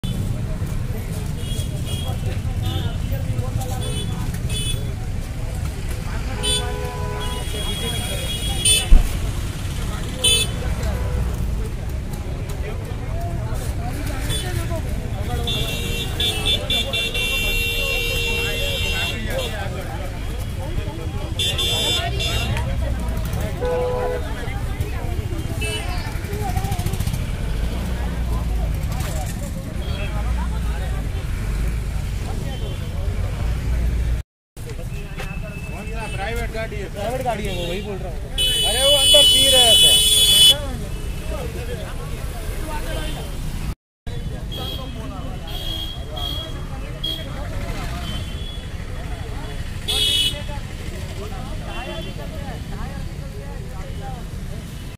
Drink and drive Accident | 2 Cars Are Beadly Crashed | at Udaipur Fatehsagar Road
2 cars accident in Udaipur Fatehsagar Road both are Face to Face 1 car driver is drink and drive and his bump into other car people in both cars was admitted in hospital and they are very serious May God Get Well Soon So You Don't Drink and Drive Ride Safe be Safe